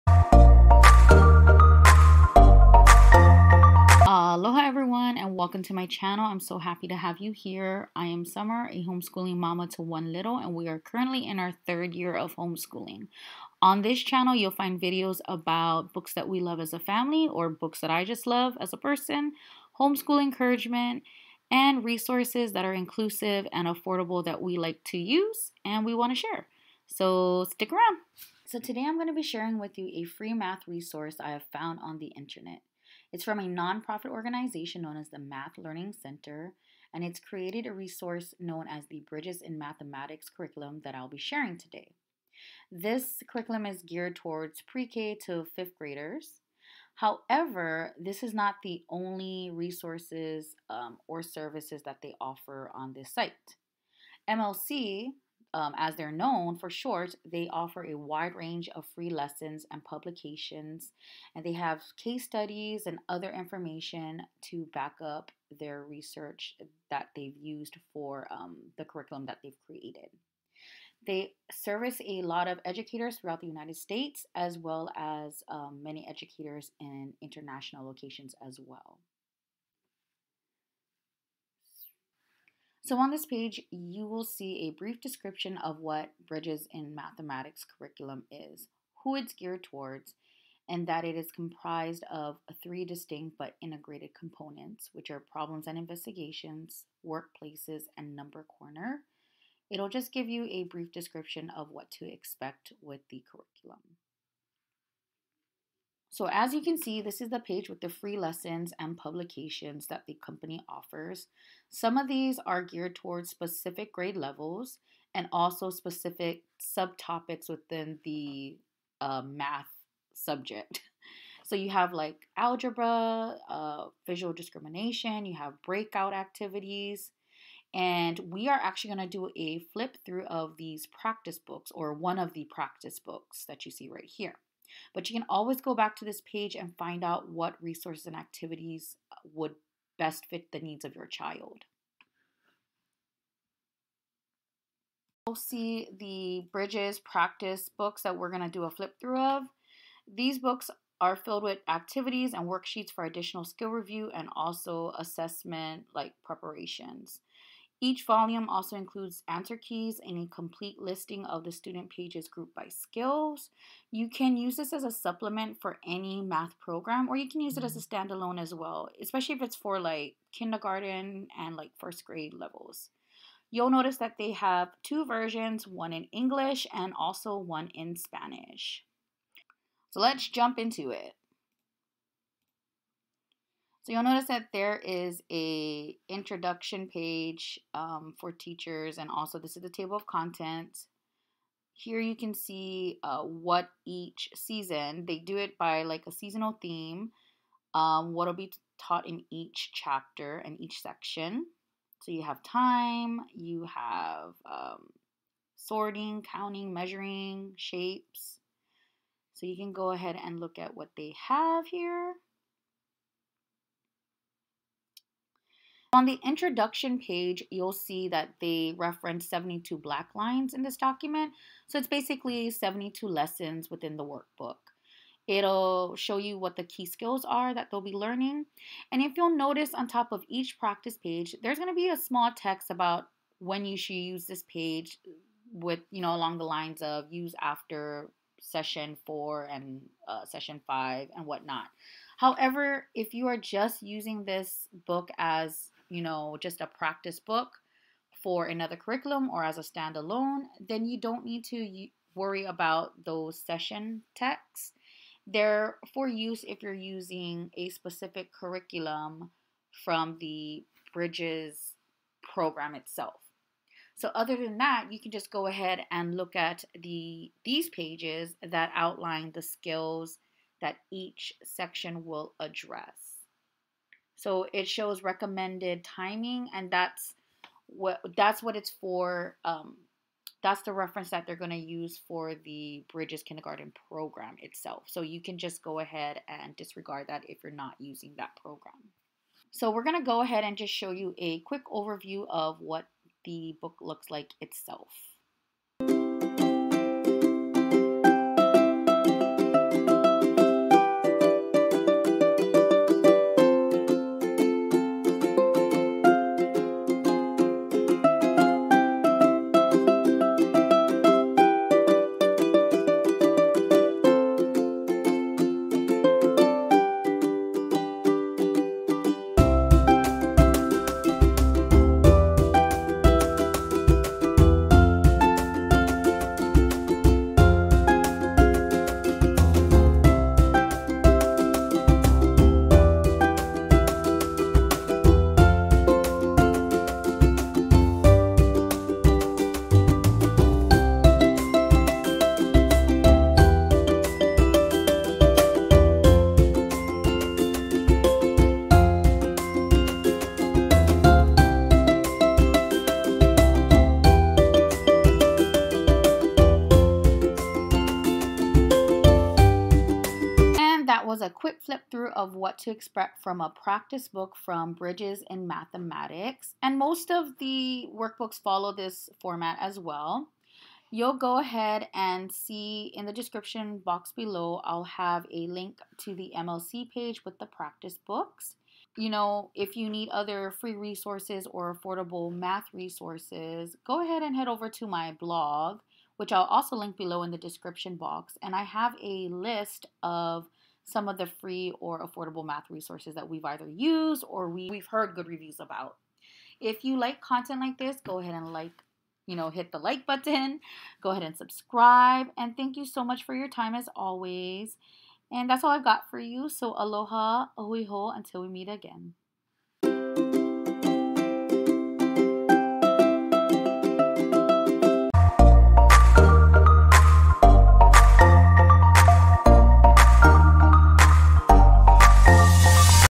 FREE HOMESCHOOL MATH // BRIDGES IN MATHEMATICS FLIP THROUGH // FREE HOMESCHOOL CURRICULUM WORKBOOK
↓↓↓ CLICK TO SEE MORE INFO ↓↓↓

Aloha! I love sharing FREE and AFFORDABLE resources that can be used by almost all homeschoolers. So here's one that I've found online for use. The resource is FREE from The Math Learning Center (MLC). The video includes a quick intro to who MLC is and then a quick flip through of the practice book.

Hope this resource is helpful to anyone who is seeking a budget friendly curriculum or supplement resource for math.

A Hui Hou (Until We Meet Again), Summer

Links: Journey of the woman post with FREE/AFFORDABLE MATH RESOURCES MLC